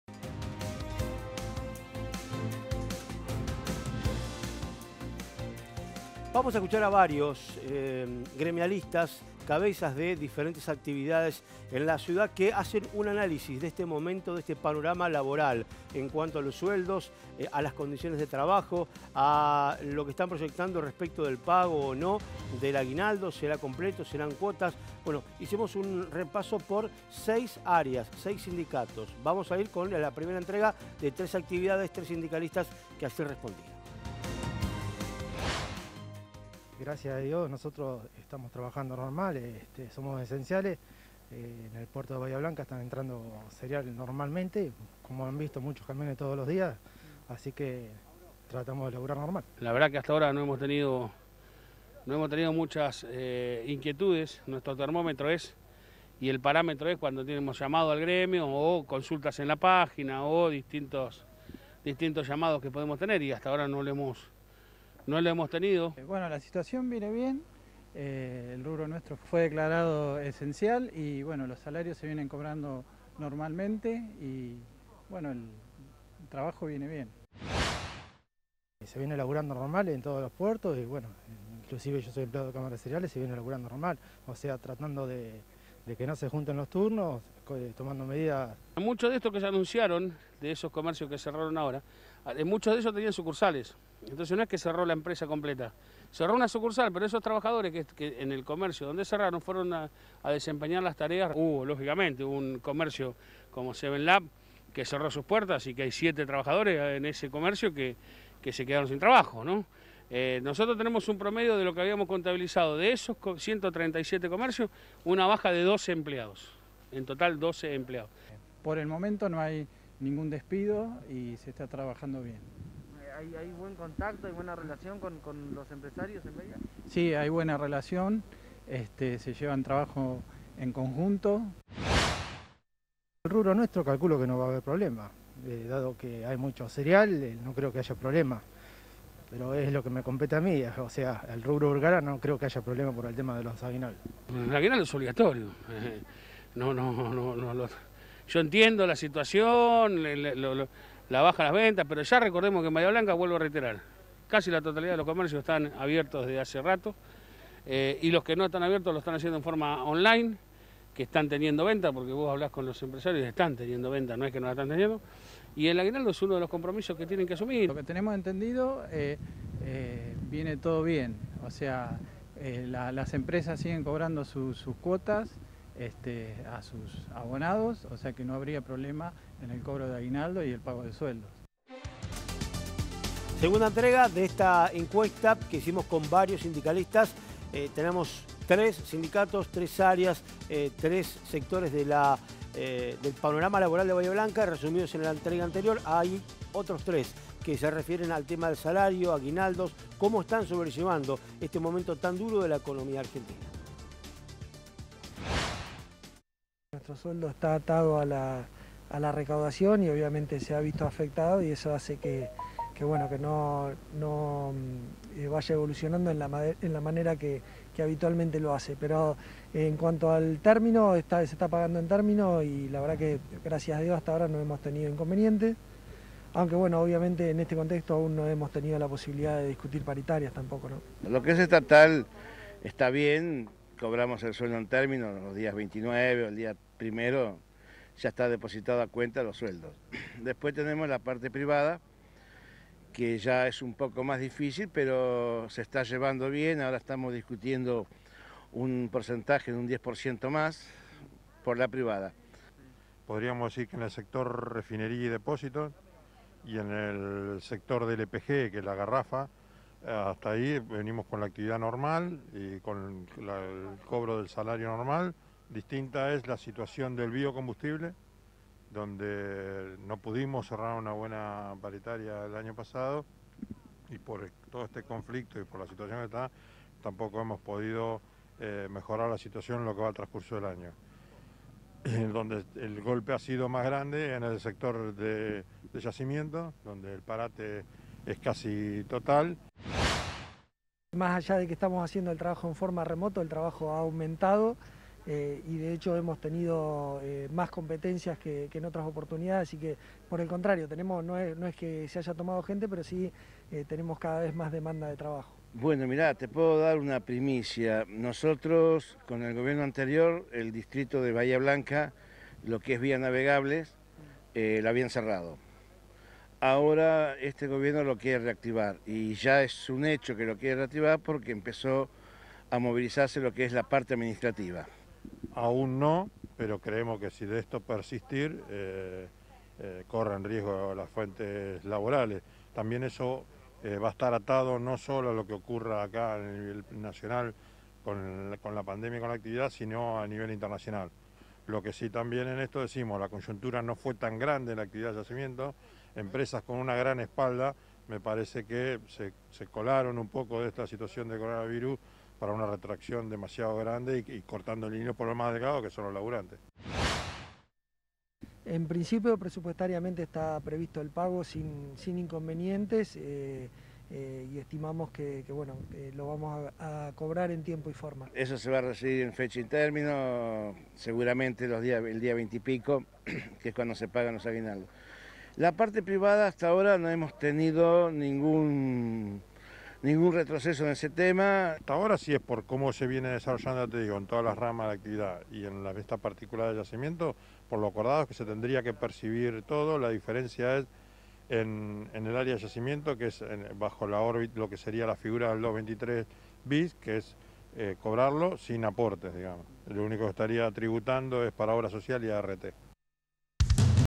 ¿Como estamos?
Seis sindicalistas analizaron el actual panorama laboral de casa sector. Hablaron de sueldos, despidos y aguinaldos entre otros temas